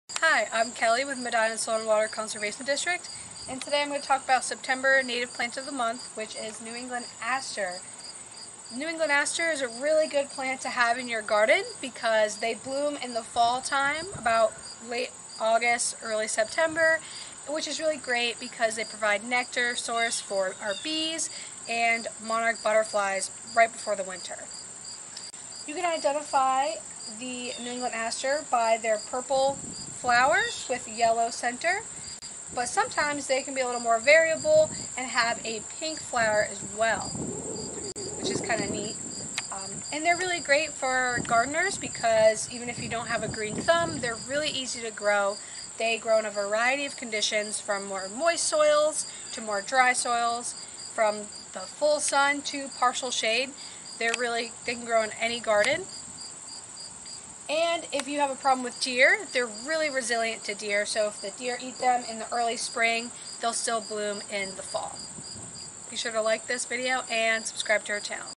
New England Asters
September’s native plant of the month is New England aster (Symphyotrichum novae-angliae). You’ll see these beauties blooming in September-October in hues of purple-pink. New England Aster can be seen growing in prairies, along streams, abandoned fields, and open areas. Its ability to grow in a variety of conditions makes it a great choice to plant in gardens around your home.